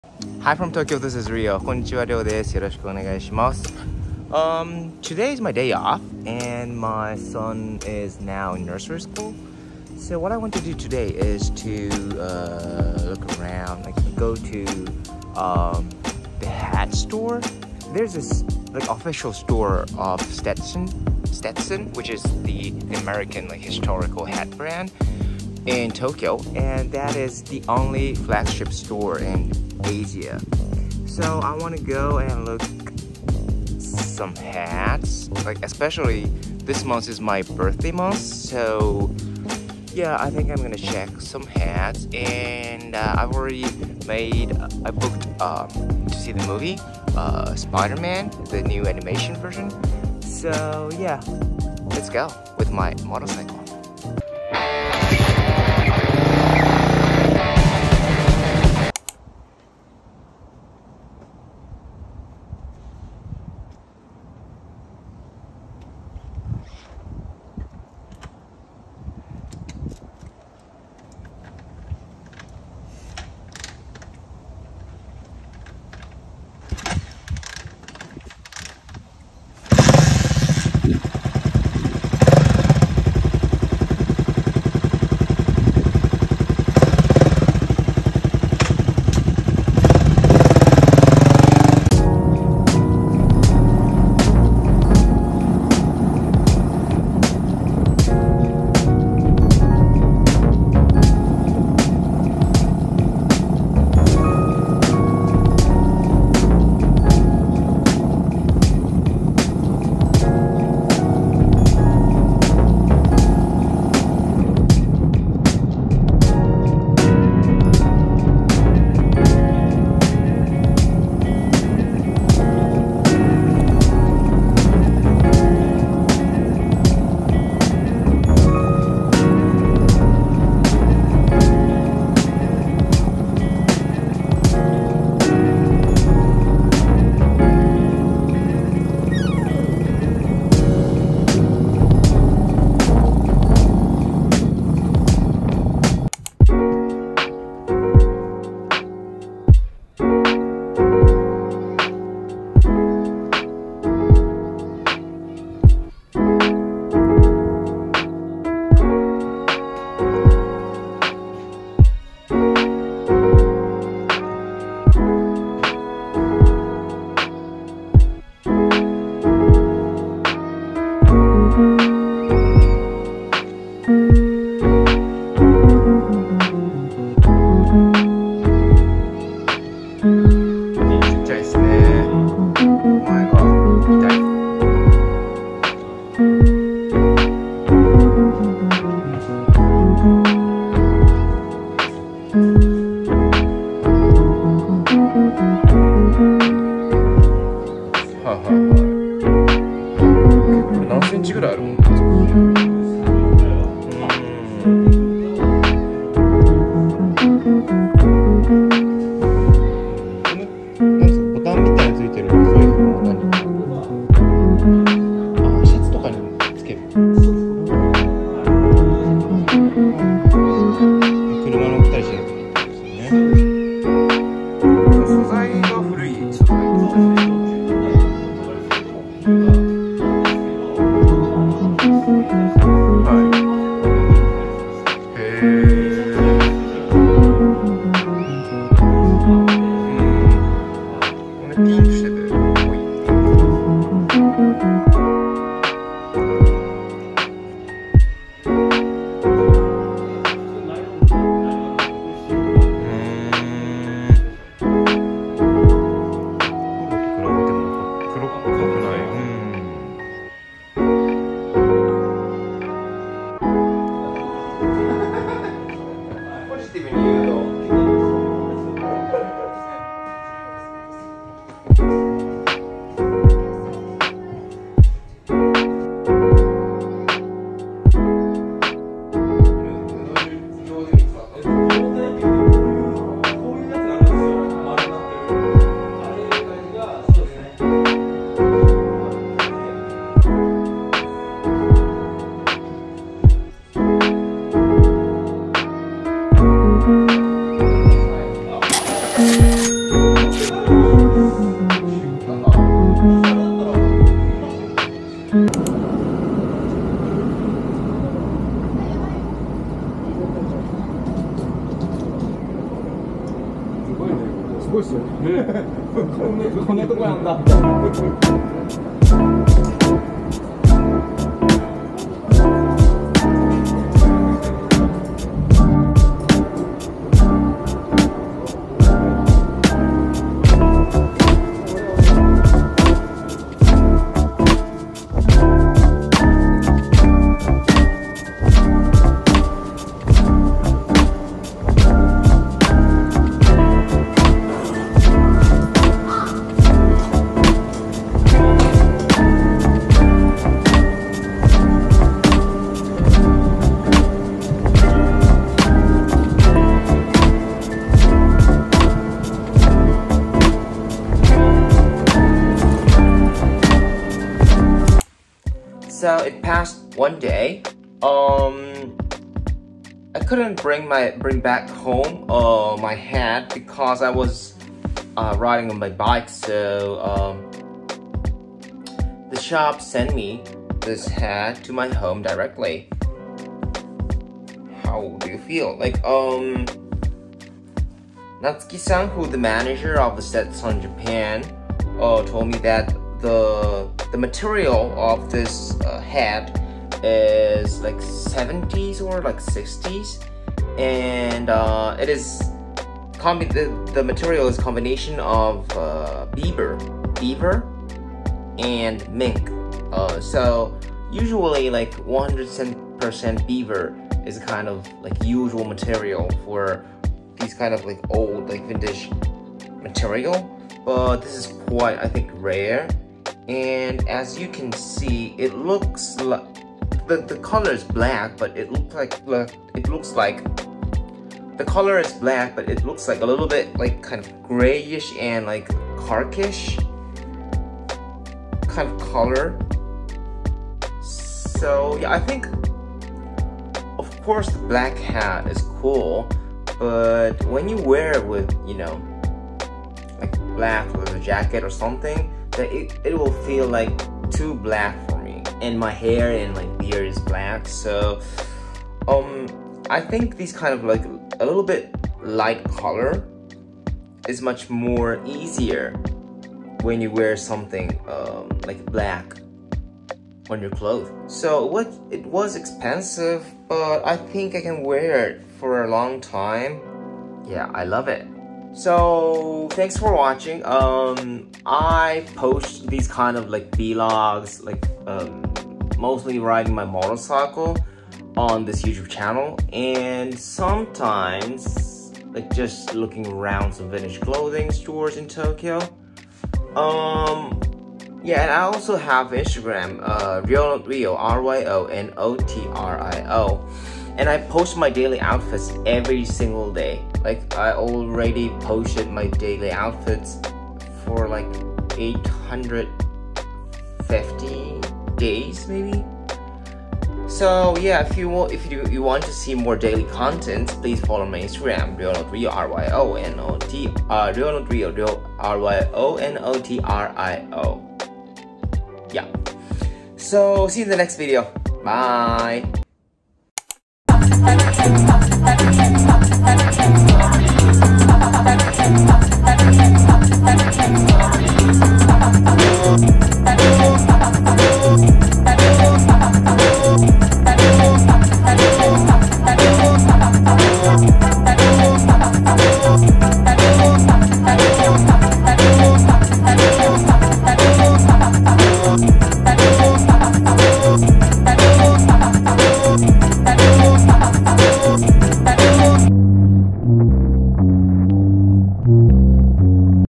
TOKYO MOTO VLOG : Hat Hunt @STETSON Tokyo
I rode my cafe racer to Stetson's only flagship store in Asia. I tried on many items and made a purchase!

カフェレーサーに乗って、ステットソンのアジアで唯一の旗艦店に行ってきました。たくさん試着して購入したのは！？
あなたが一番気になったハットはどれでしたか？コメント欄で教えてください。

OUTFIT
Tee shirt : Vintage Harley Davidson
Pants : Dickies
Belt : Chrome Hearts
Shoes : Vans
Helmet : Bell 70's RT
Gloves : found in Amazon.com
Motorcycle : Honda CB400ss Cafe Racer custom

I introduce Japanese fashion, stores, and brands from Tokyo.